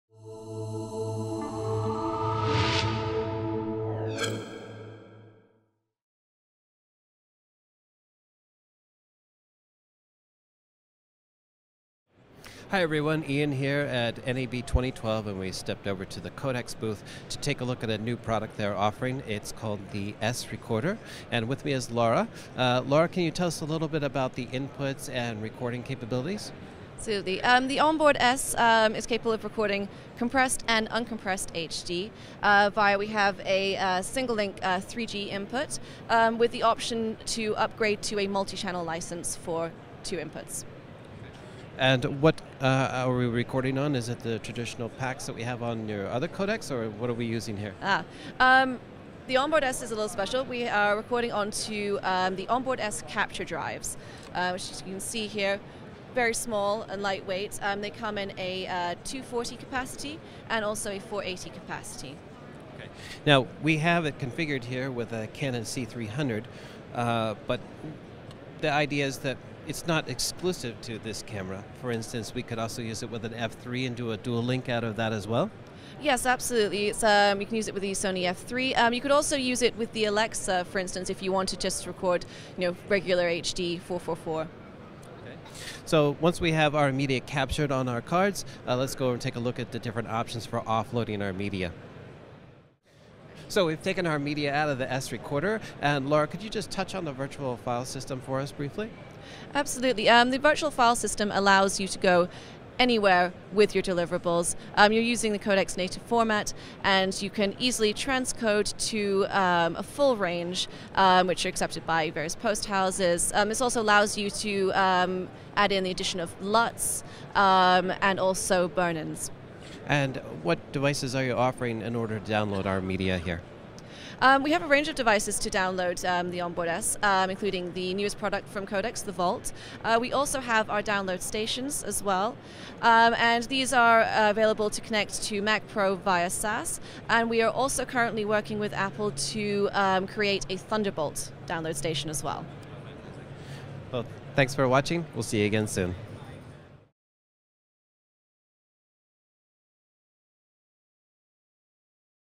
NAB '12: Codex Onboard S Recorder
The Codex Onboard S Recorder can record 4:2:2/4:4:4 10/12-bit Uncompressed HD onto a removable, solid-state Capture Drive (240GB or 480GB), as well as a single channel over 3G-SDI. The recorder is weather-resistant and weighs only 2.7 lbs, which makes it ideal for compact HD-SDI cameras like the Sony PMW-F3 and the Canon EOS C300.